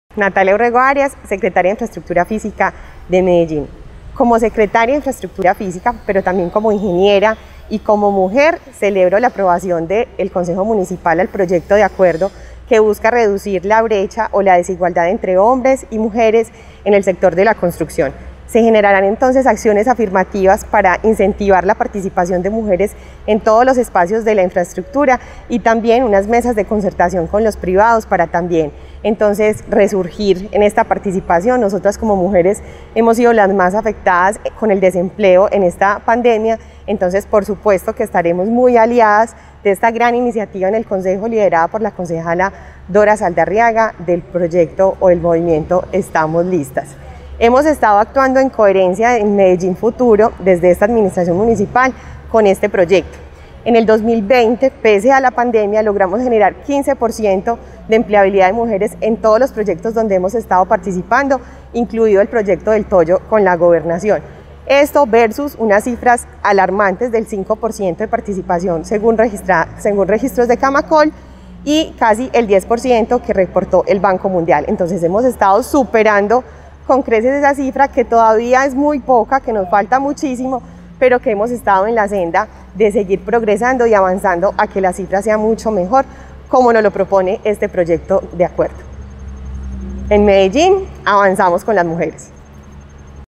Medellín supera cifra nacional de mujeres que trabajan en obras públicas
La Administración Municipal reconoce la importancia de las mujeres para avanzar en la construcción de la Medellín Futuro. Es por esta razón que, desde 2020, se promueven acciones para cerrar las brechas de género que aún persisten y vincular mano de obra femenina a los procesos de diseño, planeación, construcción, mejoramiento y mantenimiento del espacio público.